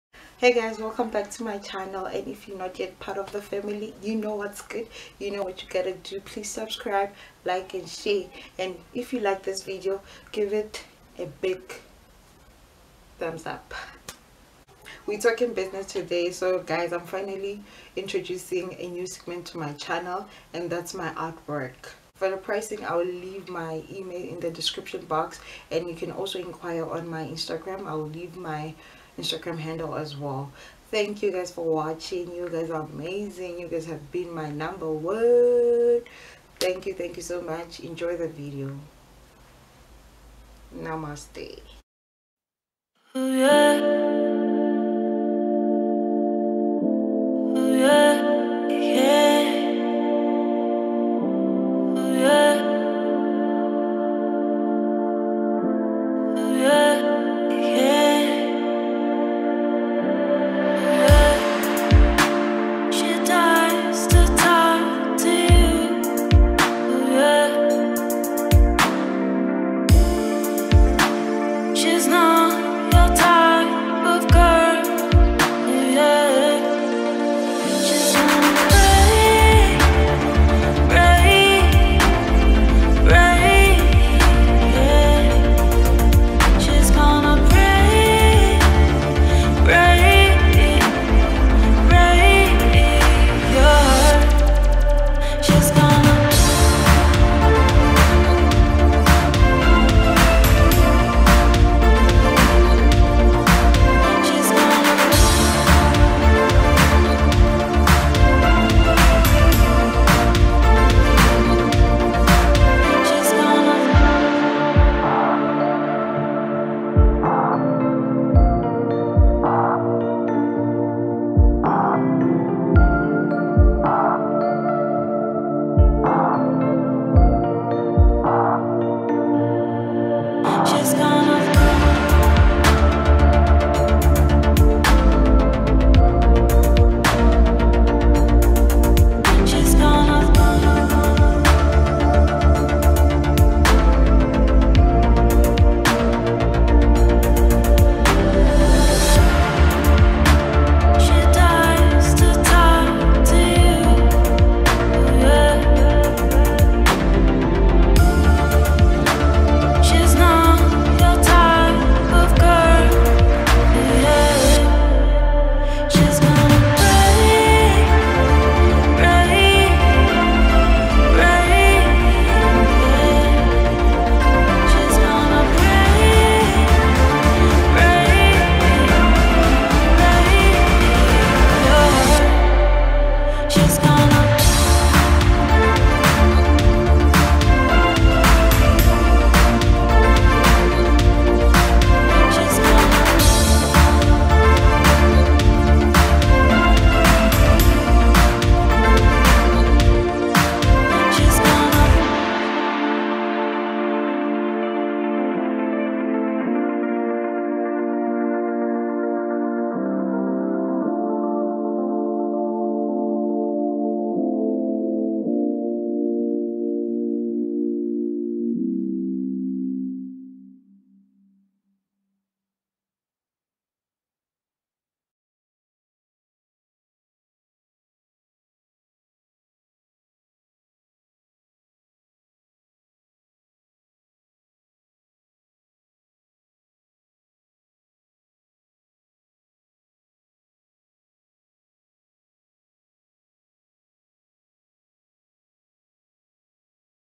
Available Acrylic artwork Showcase//South African youtuber
Hey guys hope you well and safe. i am introducing my artwork to the channel...all the artworks are available for purchase...enjoy the video